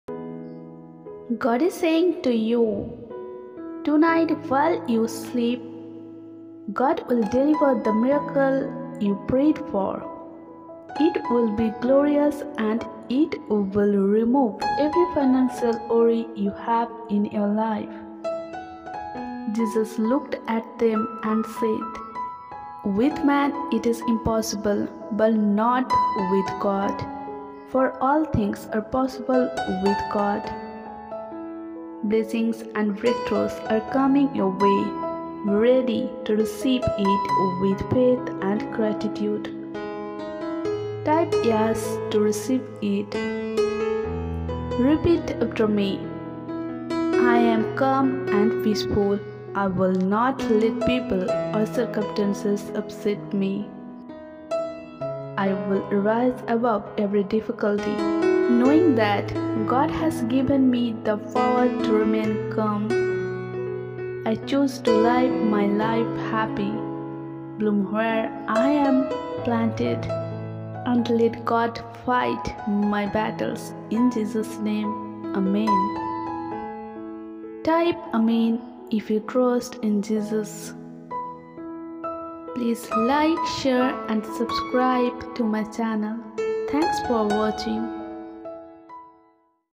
God Has Sent This Message 📩 To Fulfill Your Wish...❣️ | God msg
Inspiring and uplifting God quotes from people of all walks of life and people who at difference crossroads in their journey with Him. Let these quotations remind you of His character of love and mercy. Our life is not without trials but God gives us strength to face the challenging times. His Love for us is beyond words and He will deliver us from any pain or anguish we go through, we must only let Him in. God has helped me through all of my life's problems, he hears my prayers everytime i need Him, and for that i could never thank Him enough. When my time is up on this beautiful earth I look forward to being embraced in His love in heaven as he has given me on earth.

Universe message
Jesus message
Angels message
Jesus message
Life quotes
Jesus quotes
Faith quotes